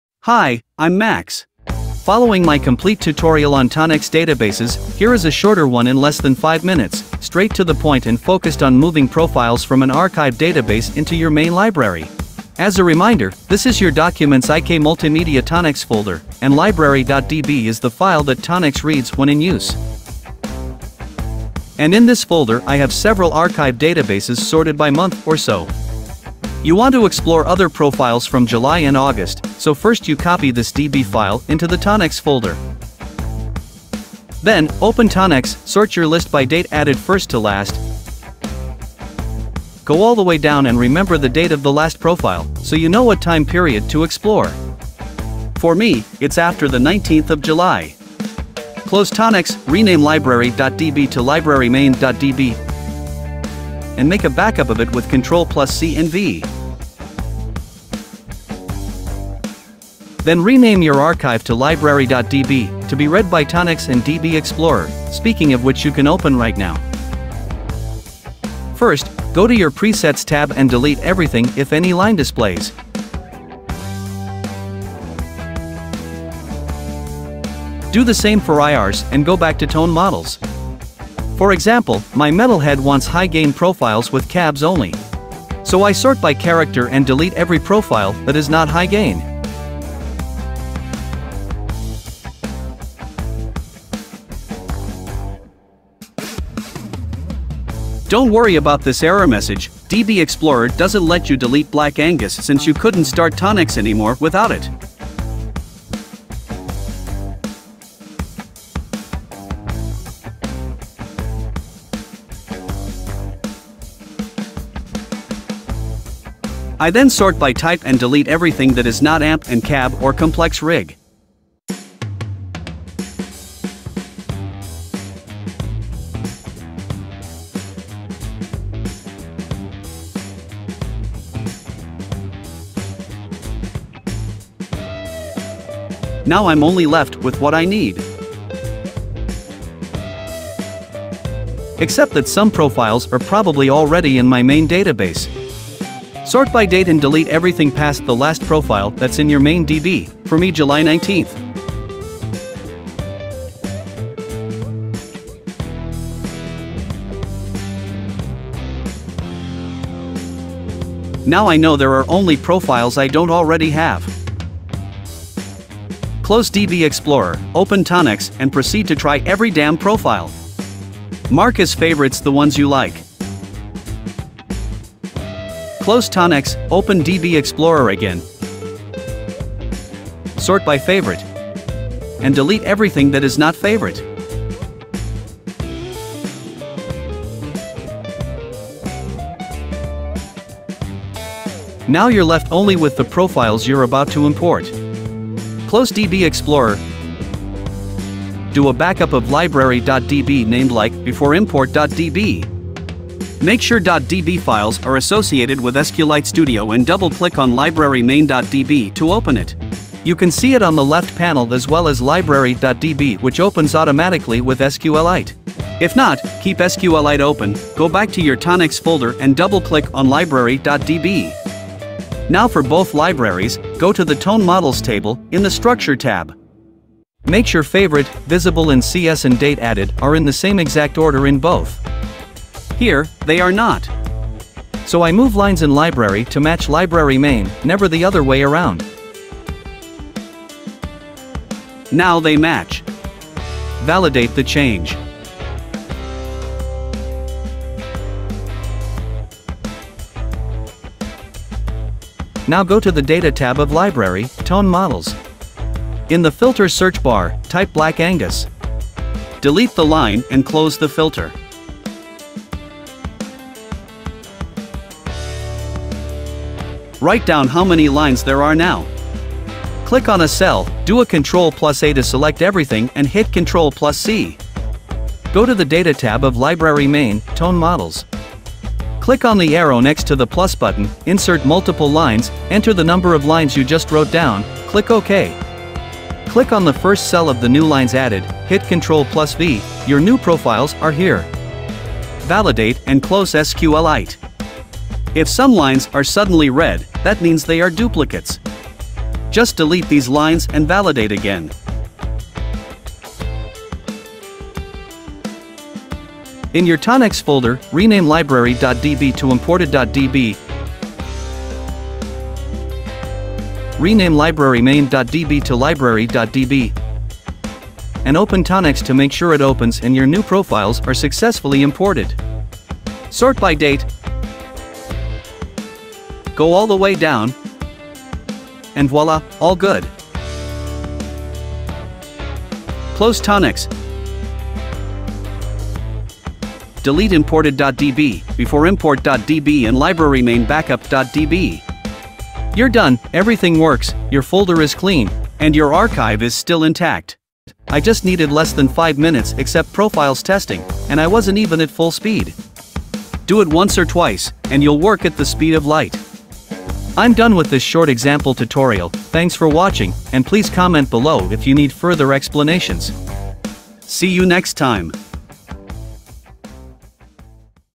TUTORIAL - ToneX DB transfers done quick
Following my extensive tutorial, here is a shorter, straight to the point version with less side explanations, specialized in moving profiles from an archive database to your main library.

00:00 Intro
00:29 Preparation
01:07 ToneXdb Explorer
02:45 Selecting profiles to import
03:17 SQLiteStudio
05:32 Final cleanup
05:48 Conclusion